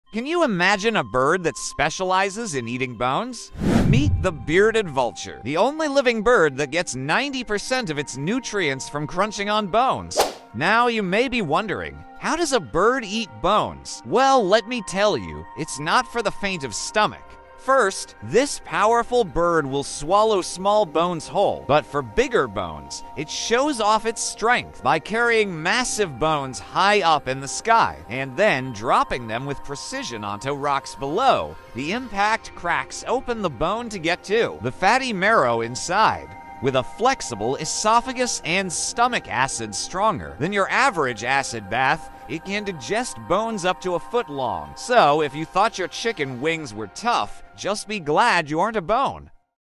The bird that feeds on bones: Bearded Vulture(Bone Eating bird)
Here are some key facts about the bearded vulture:

- Scientific name: Gypaetus barbatus

- Also known as the lammergeier, it is a large old-world vulture found in Europe, Asia, and Africa.

- Has a wingspan of up to 2.5 meters, making it one of the largest flying birds in the Old World.

- Features a reddish-brown naked head and neck with a ruff of grayish down around the neck that gives it a "bearded" appearance.

- Feeds almost exclusively on bones by dropping them from heights or carrion left by other predators. Its acid stomach digests bones.

- Plays an important ecological role in recycling nutrients from bone and helping control small mammal and carrion populations.

- Nest nests on inaccessible cliff ledges and caves, often over long periods which improves nesting habitat for other species.

- A long-lived species with pair bonds that can last decades. Late maturity and low reproduction make it vulnerable.